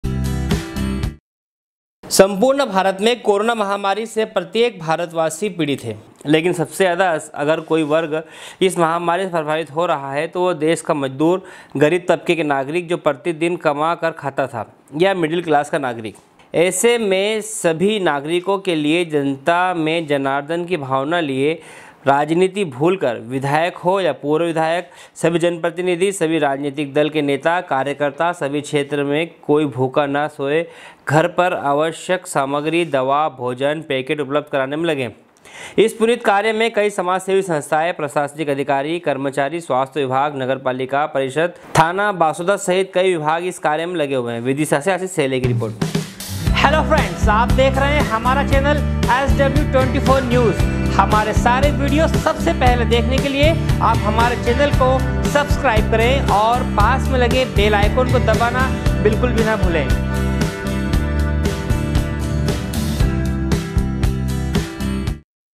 SW 24 News Madhyapradesh vidisha #20.042020sw24news
सम्पूर्ण भारत मे कोरोना रूपी महामारी से प्रत्येक भारत वासी पीड़ित है लेकिन सबसे ज्यादा अगर कोई वर्ग इस महामारी से प्रभावित हो रहा है तो वह है देश का मजदूर गरीब तबके के नागरिक जो प्रति दिन कमा कर खाता था या मिडिल क्लास का नागरिक
विदिशा से आशीष सहेले की रिपोर्ट#sw sw24news sw24newsmadhyapradesh livenews news24 SW 24 News Madhyapradesh vidisha #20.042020sw24news